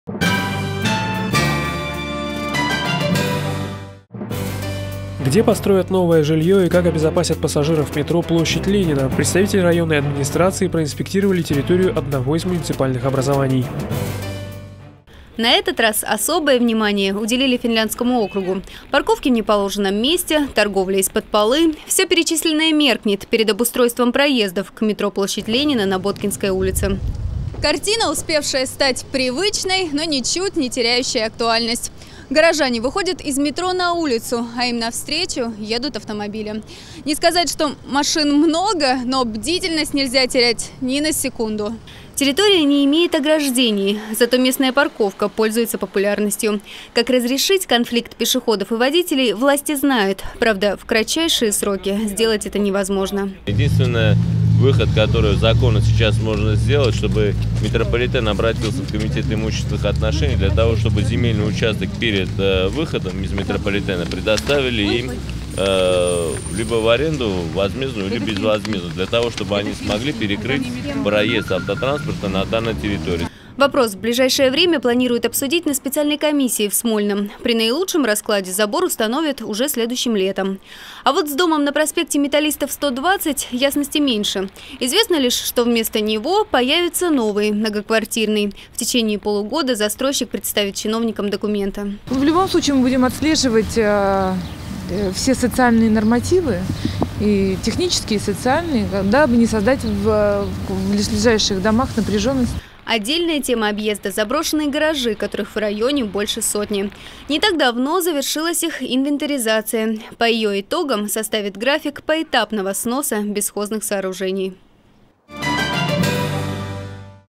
Где построят новое жильё, и как обезопасят пассажиров ст. метро "Площадь Ленина"?
Представители районной администрации осмотрели территорию одного из муниципальных образований.
tkt-tv.tv